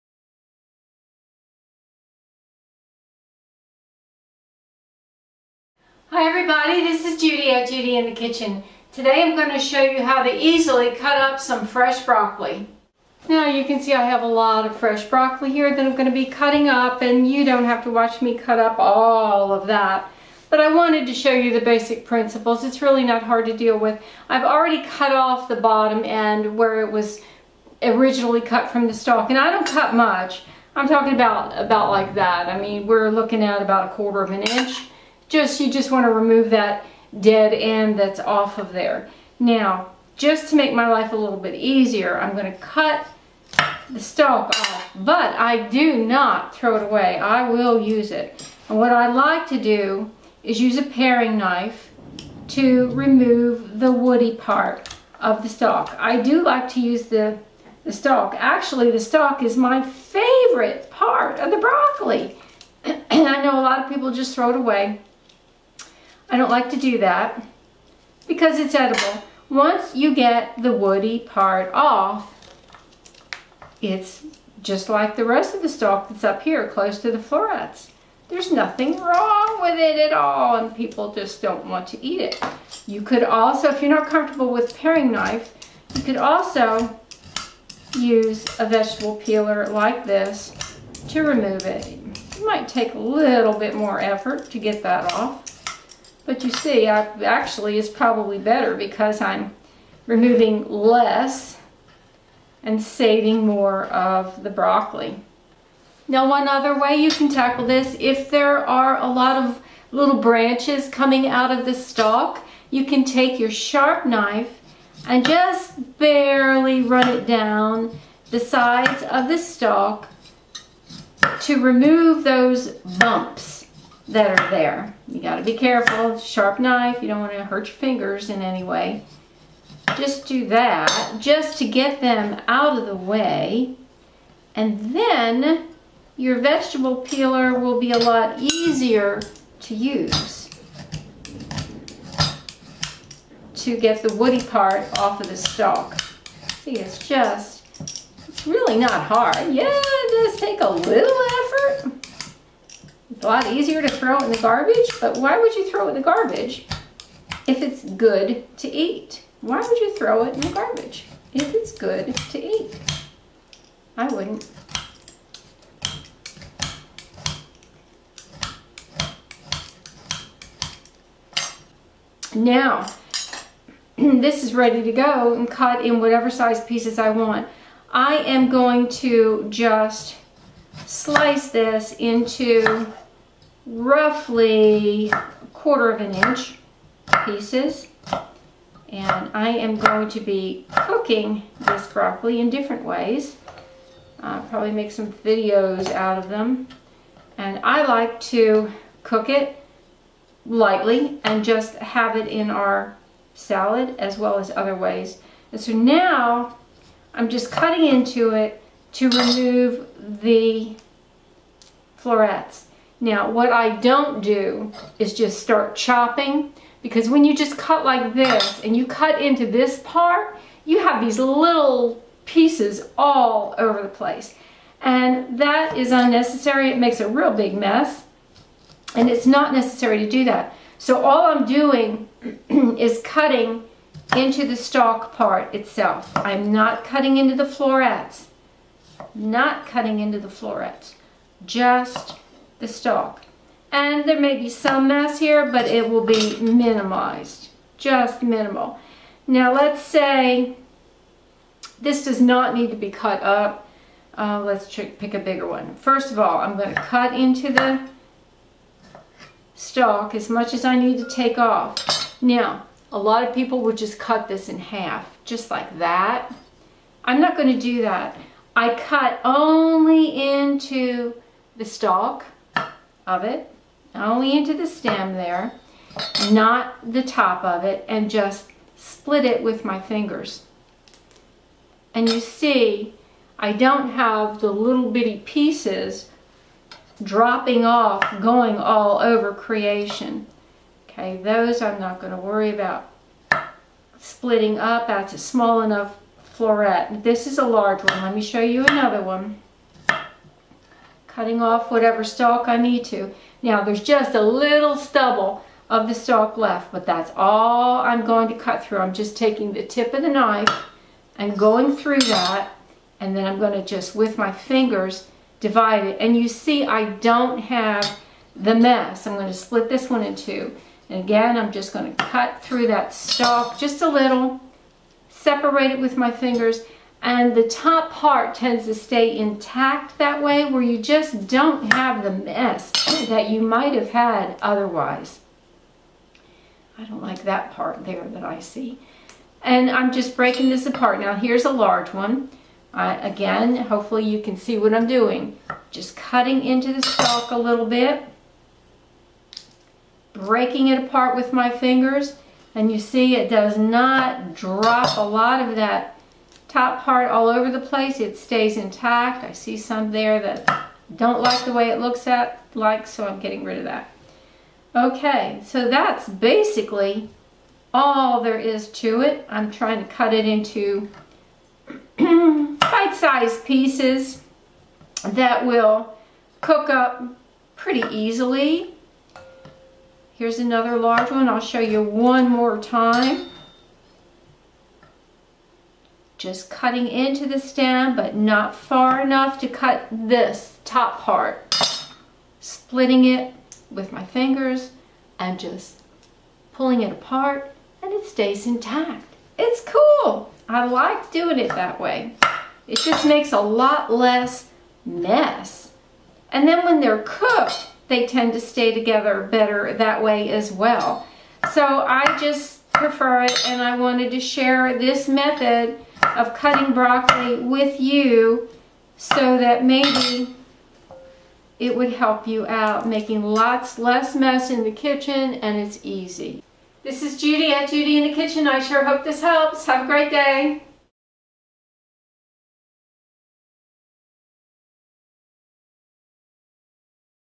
Easily Cut Fresh Broccoli with Less Mess
In this video I show how to easily cut fresh broccoli without so much mess from the florets.